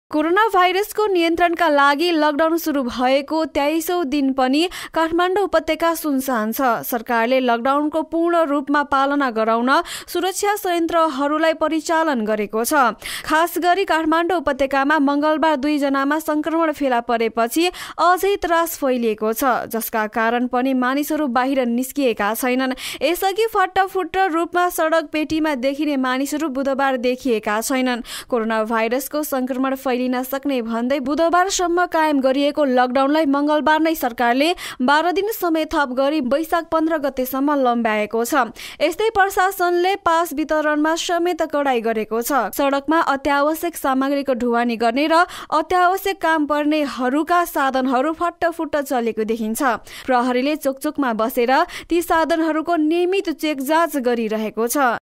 लकडाउनको २३ औं दिन
लकडाउनको २३ औं दिन, काठमडौं उपत्यका सुनसान